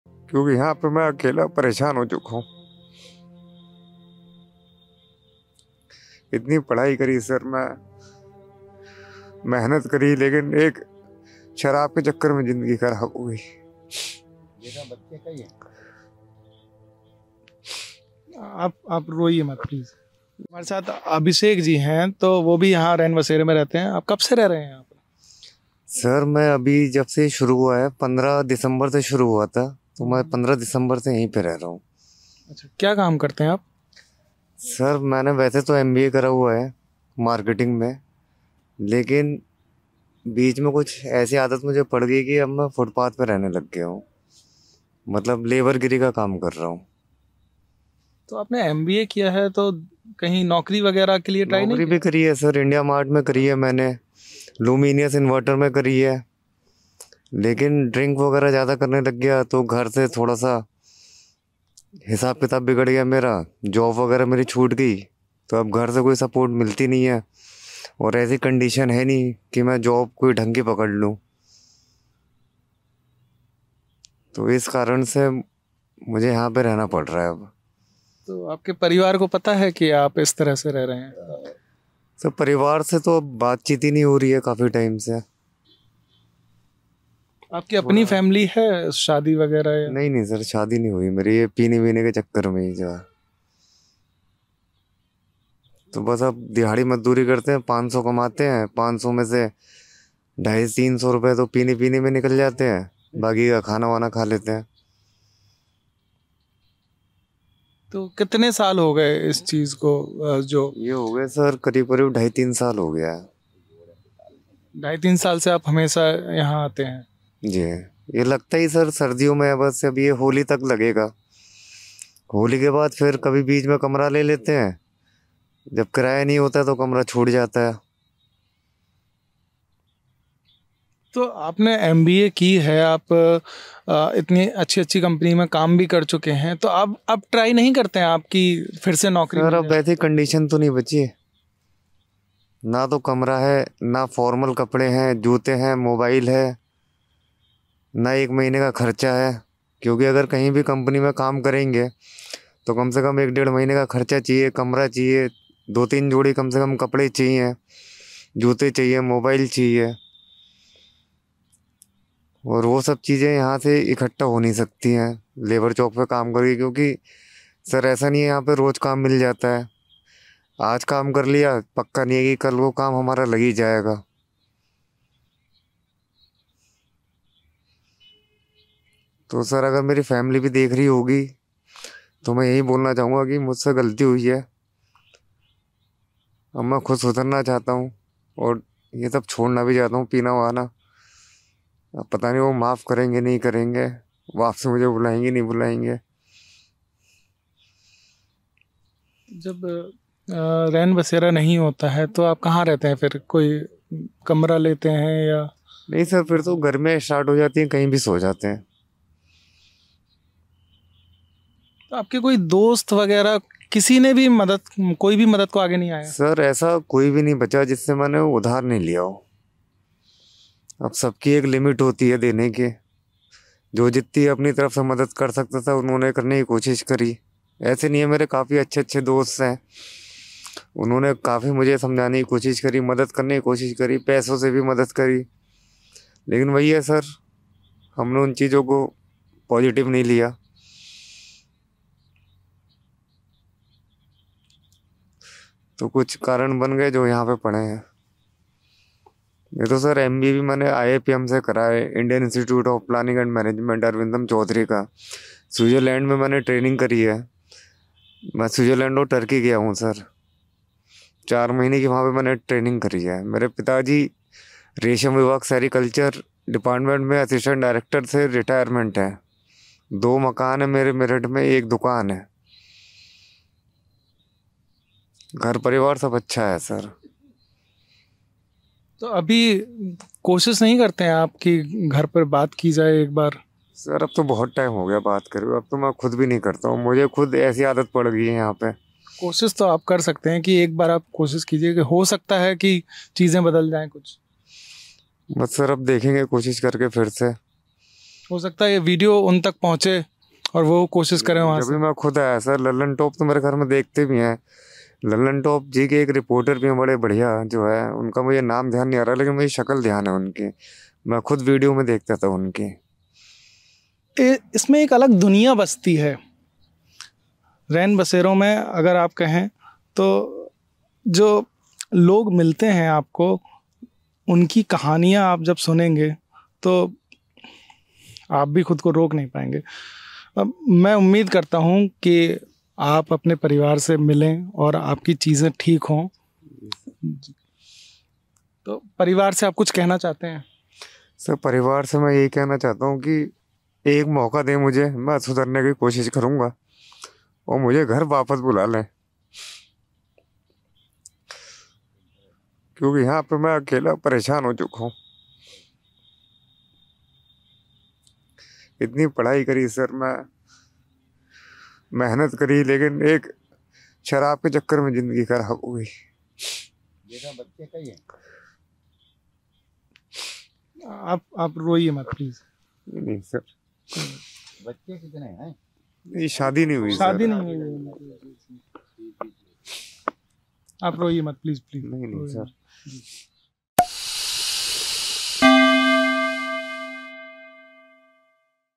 दिल्ली के रैन बसेरा में MBA पास शख्स आप बीती बता गया, फिर Lallantop से जो कहा, कहानी भावुक कर देगी
We are troubled by the harsh cold in North India. But there are many people who are hoping for a shelter in this cold. For them, government night shelters are the only shelter. The Lallantop's team spoke to some such night shelters and talked to the people there. Found a person in a night shelter who has done MBA and has also received training from abroad, but why is he forced to live in a night shelter today? Watch the full report in this video. Watch now!

Edited By: Prashant